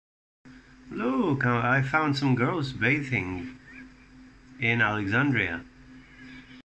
Topless girls at the beach!
I was riding my horse along the beach in Alexandria and I came accross these ladies relaxing at the beach. Talk about realism!